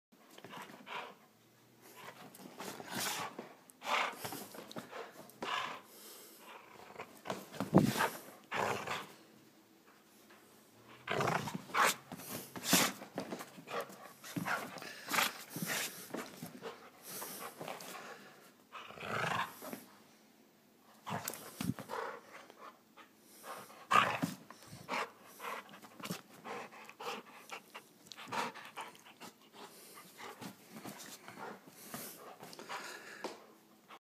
Amstaff Needs to Tug
Apoc tugs on rope and it's all for fun.